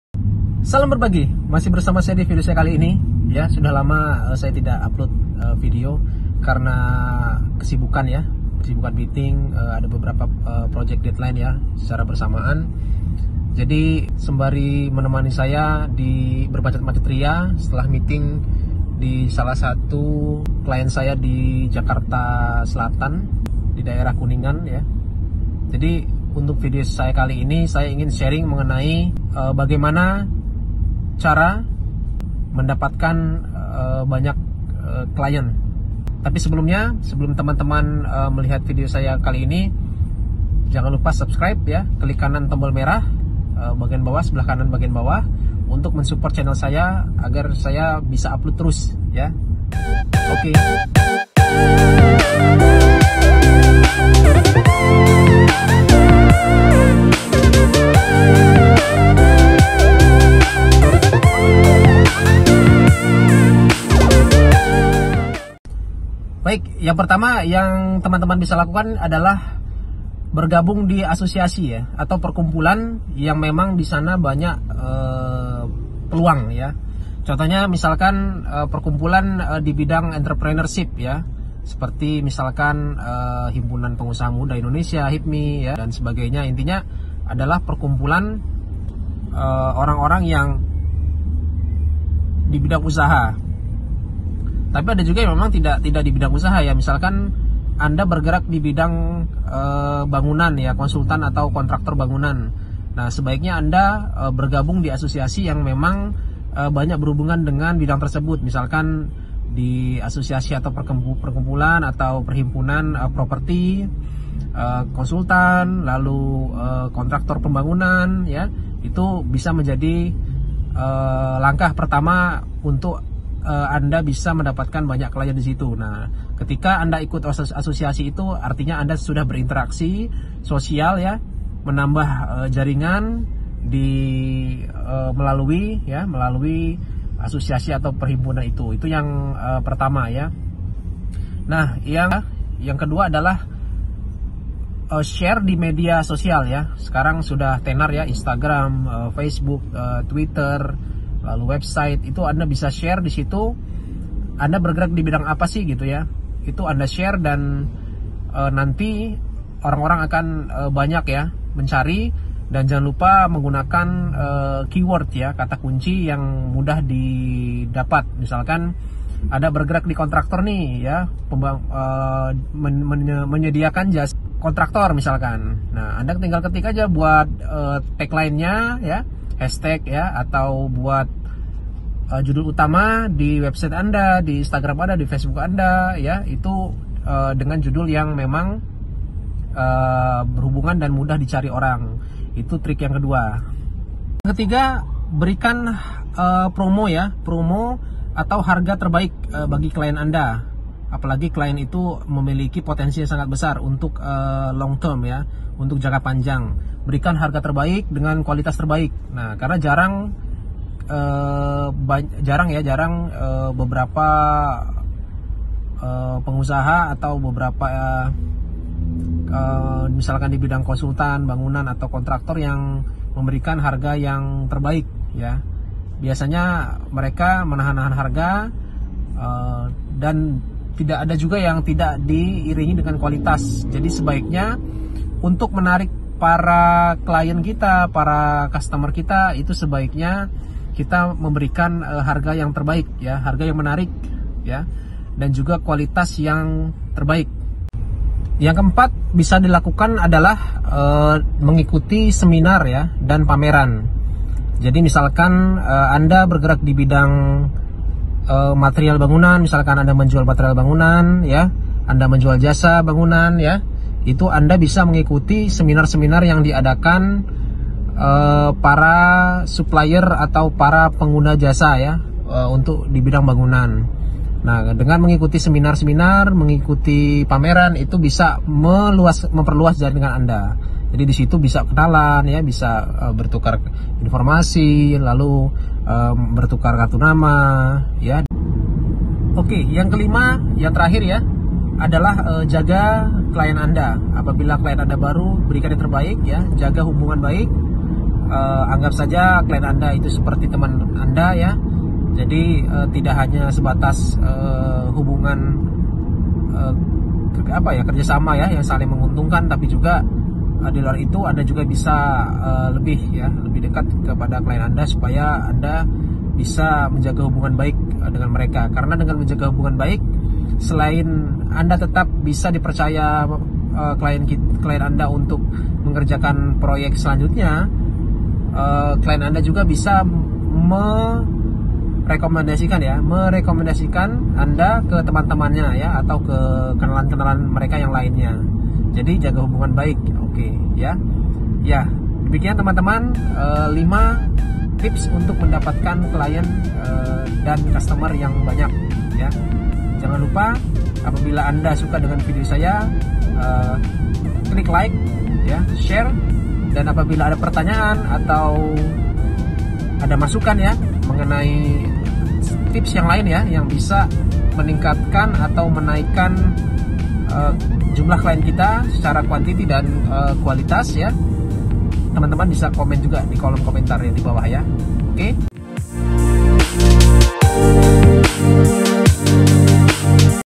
Cara Mendapatkan Banyak Client 👍🏻
Salam berbagi 🙏🏻
sudah lama rasanya saya tidak share video
kali ini saya mengshare video, tentang cara membangun relasi yang baik untuk mendapatkan banyak klien...